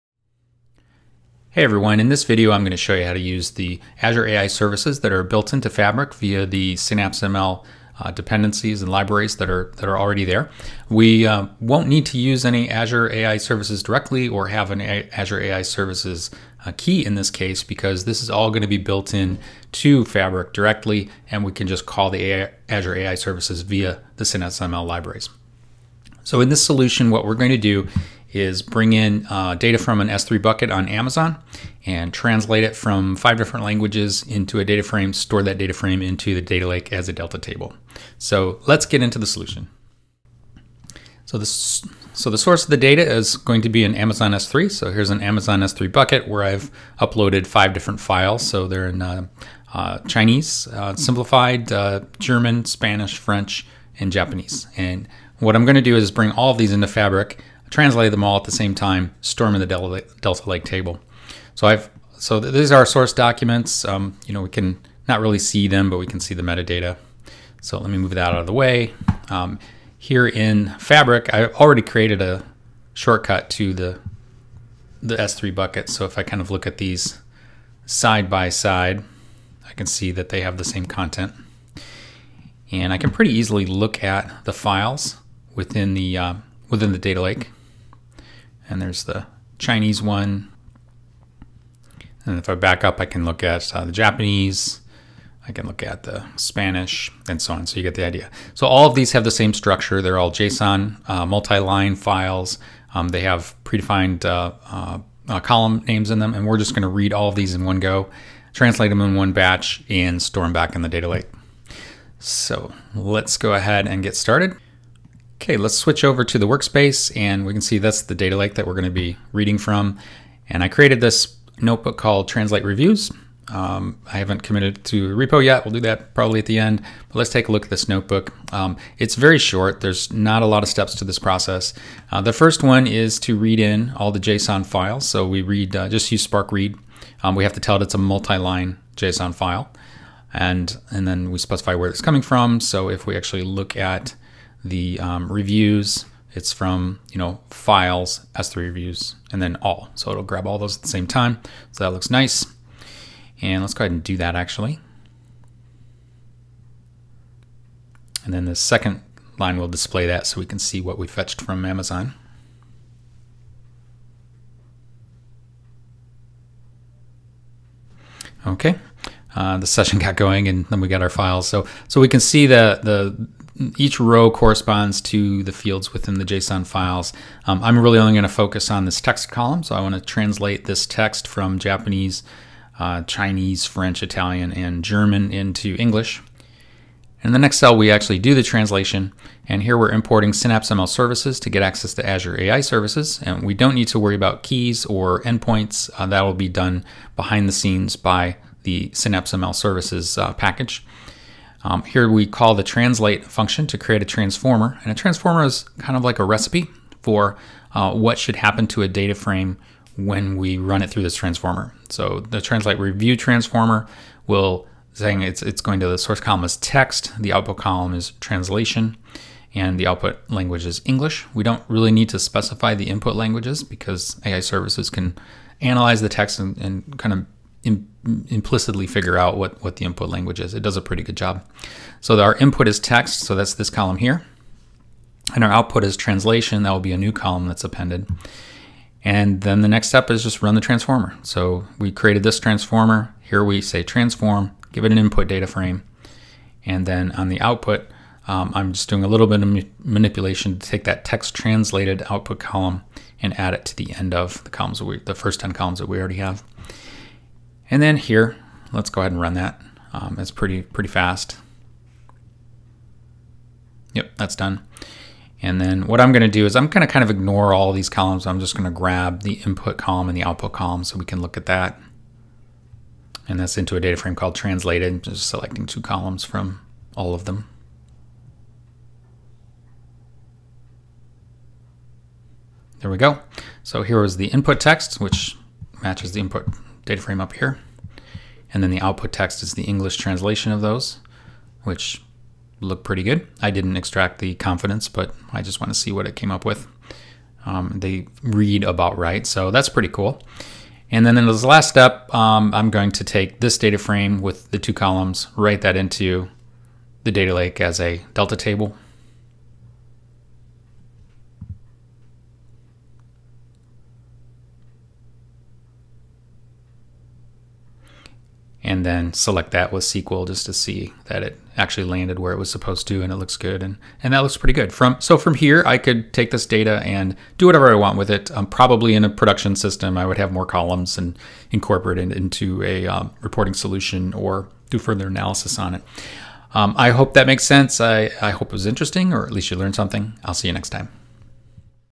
Incorporating Azure AI Translation into a Fabric Data Lake Solution
Using Fabric's embedded Synapse ML features, we can leverage Azure AI services directly in Spark notebooks to enrich and transform data using Language AI Services. Learn how to use these powerful services using a Python Jupyter notebook stored in a Fabric workspace.